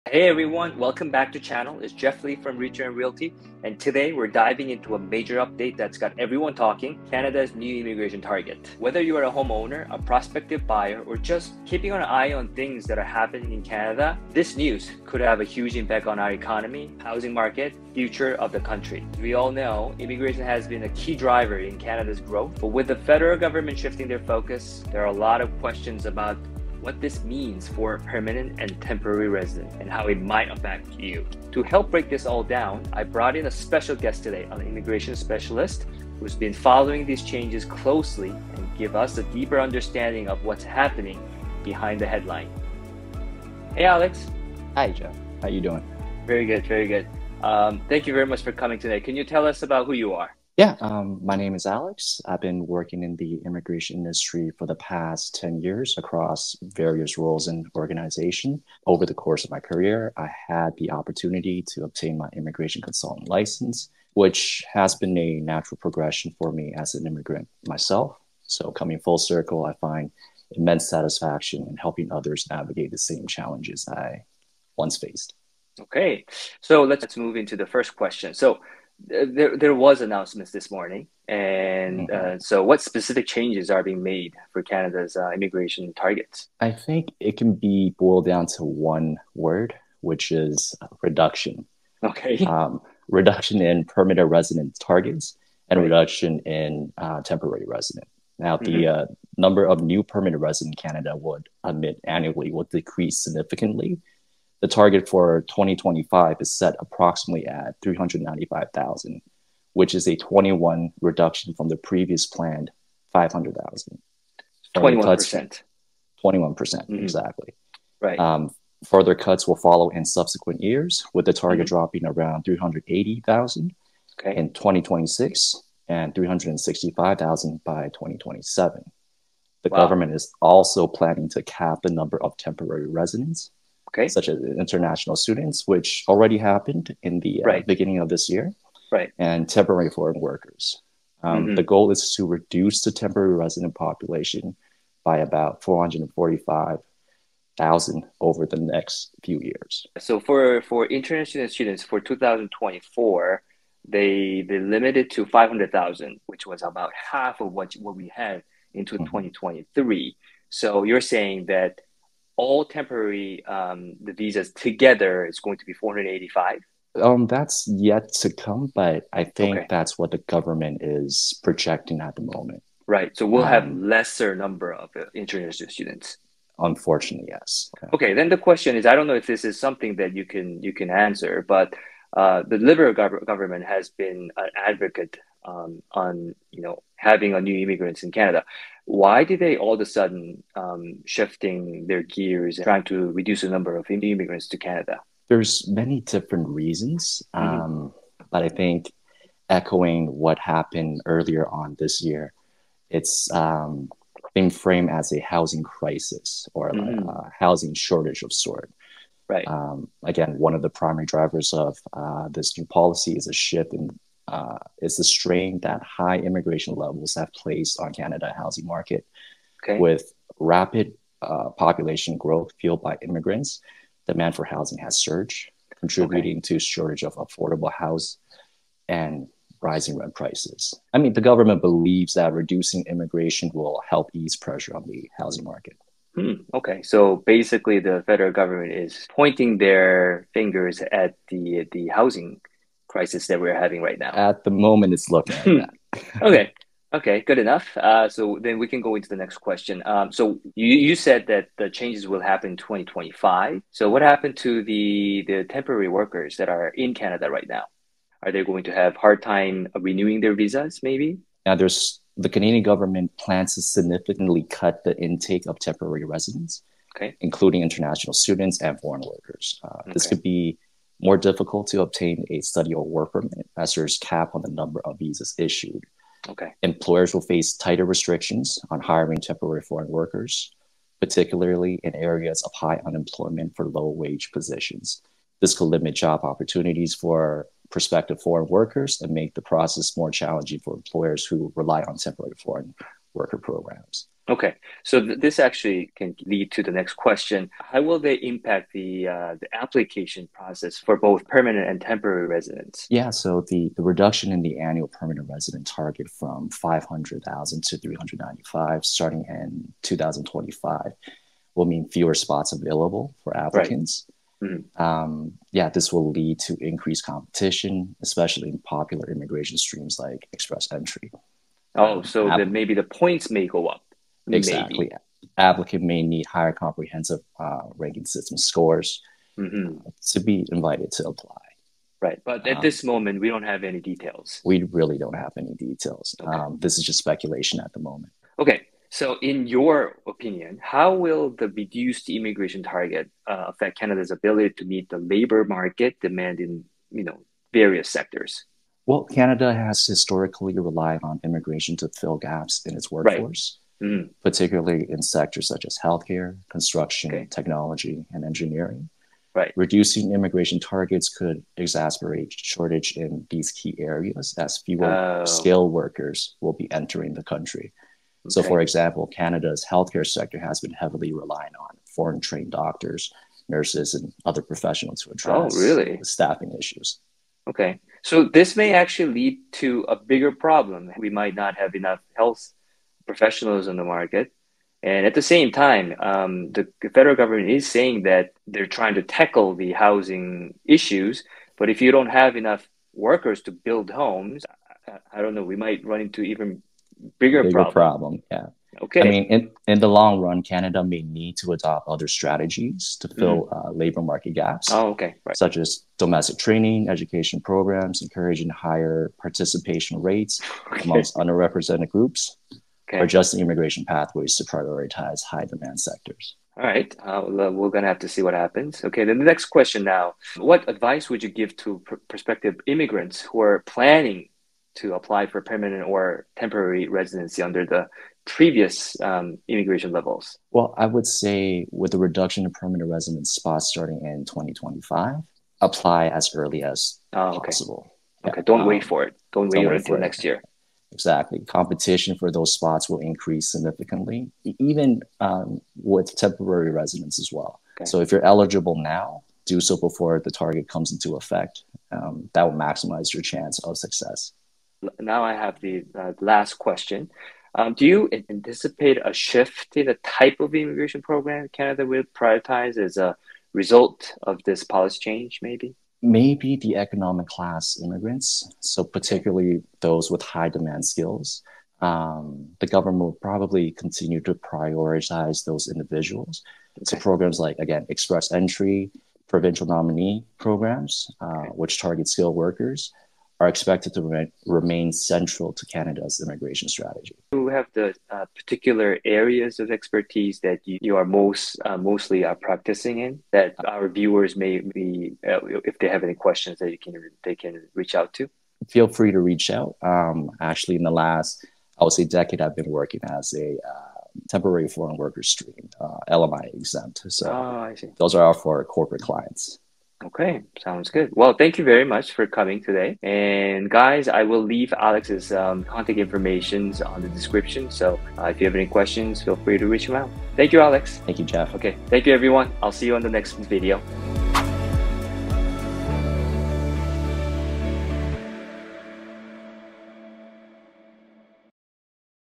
[ENG] CANADA IMMIGRATION PLAN FOR 2025 - 2027, EXPLAINED
In this video, we dive into Canada’s recent immigration plan for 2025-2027, which introduces a significant 21% reduction in new permanent and temporary residents. We’ll explore what this policy shift means for Canada’s economy, housing market, and job landscape. Whether you’re curious about the implications for the housing market, the labor force, or you’re just following Canada’s evolving immigration policies, this video covers it all! Tune in to get insights from industry experts and learn how these changes might impact newcomers, residents, and the country’s future.